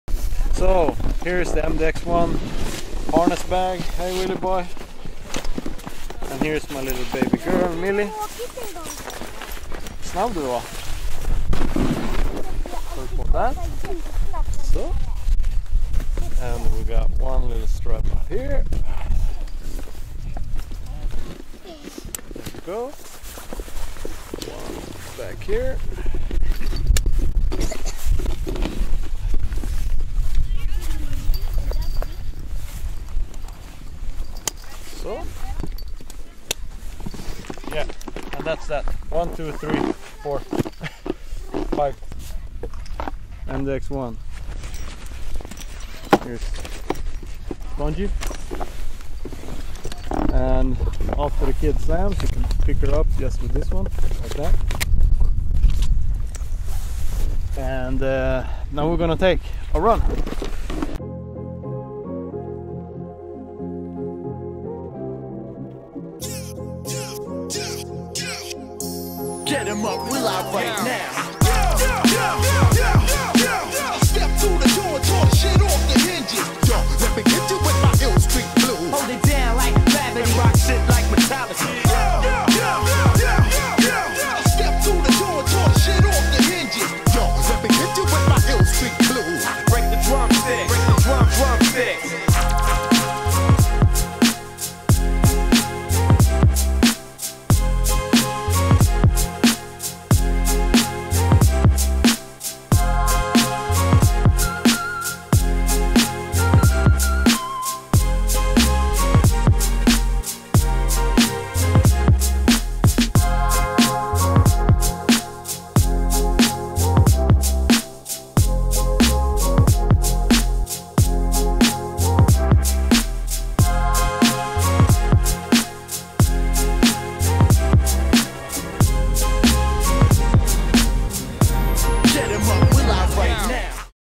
MDXONE snowbard harness try out with Millie Alund presented by Hazeland
Millie and Hans Alhund try out the MDXONE ski & snowboard harness first day of the season. Conclusion  is that the harness is great for lerning kidz to ride and its fenomenal to get parents to take real laps with your kids from day one onboard!